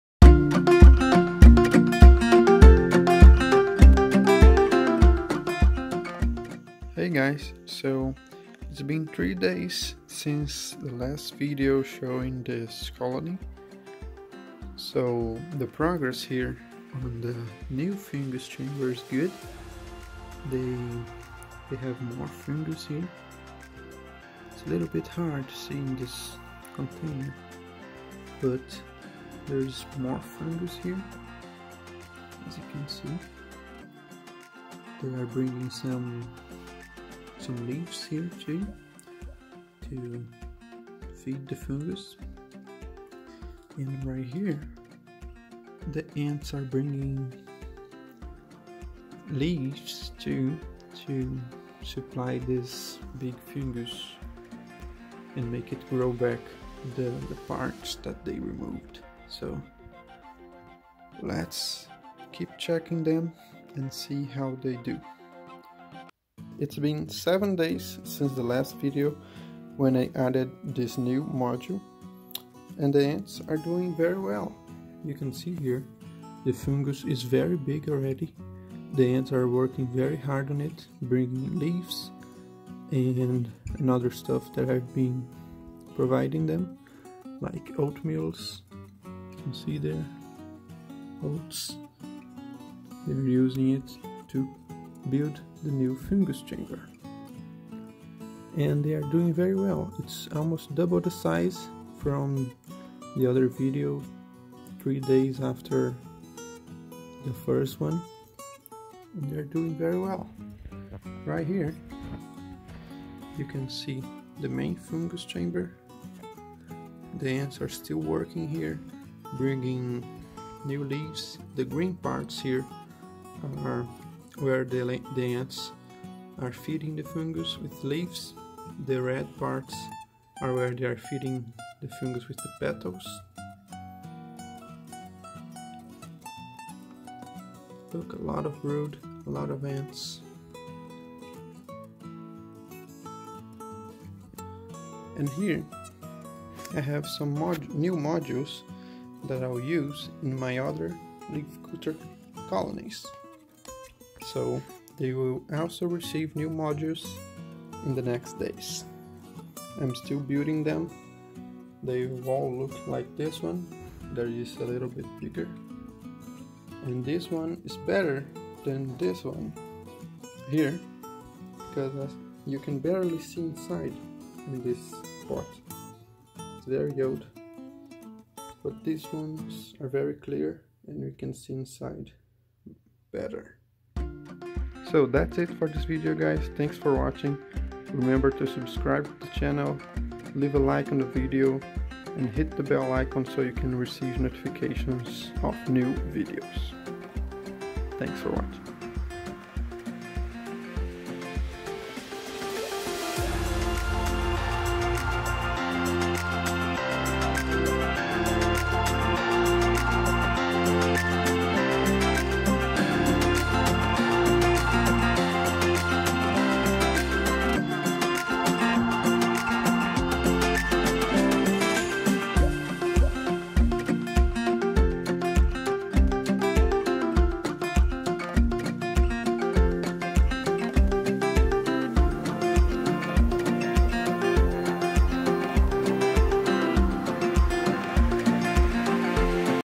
THE FUNGUS IS GROWING! - LEAFCUTTER ANT COLONY
Since the last video about this colony the fungus have been growing and in this vídeo I show you the progress of this leafcutter ant colony.